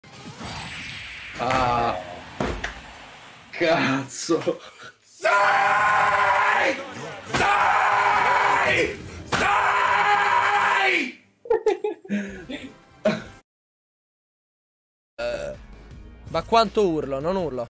TWITCH ITALIA IN A NUTSHELL
Blur che ragequitta e Marco bestemmia